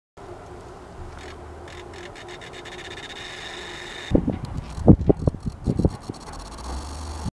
Junkyard Car Clock Test
Hooking up a small 12-volt cell to Ford Five Hundred and Chrysler 300M clocks in the junkyard.